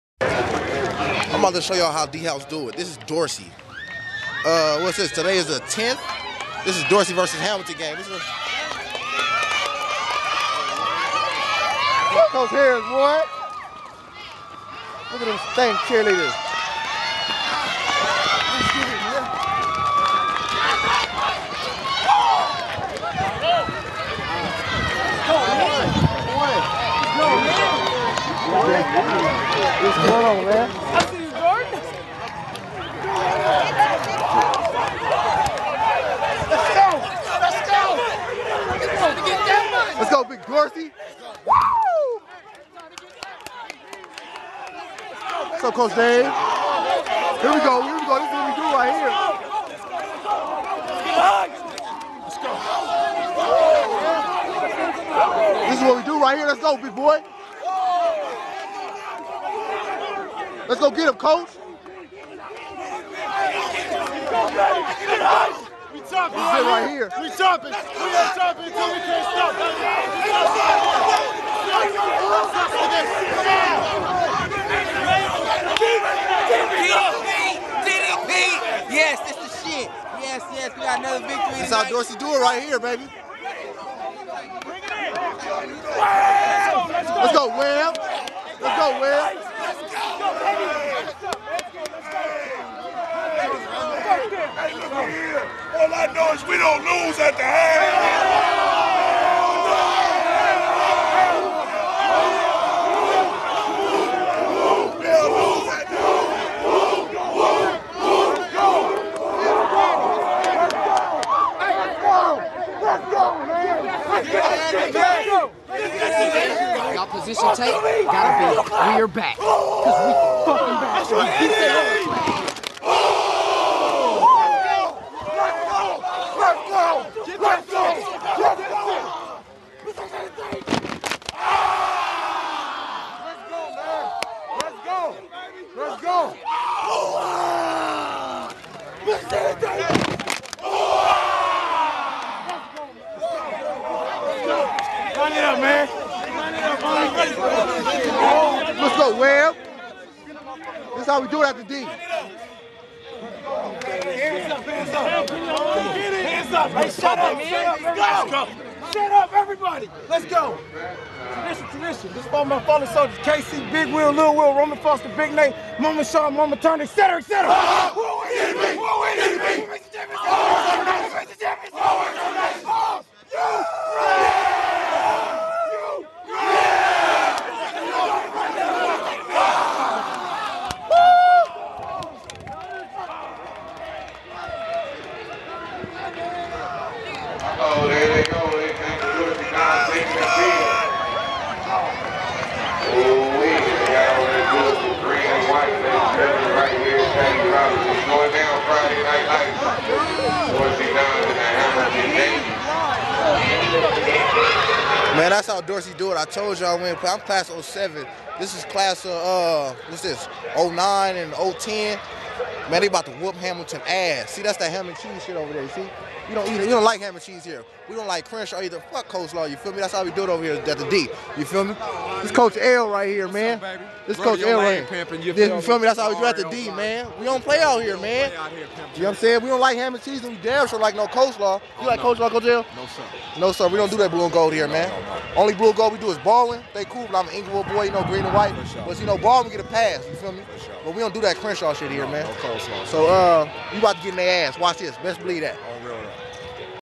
Dorsey football 2 beats 2 claps vs hamilton
As dorsey takes the field we got us a little pre game tradition. take notes.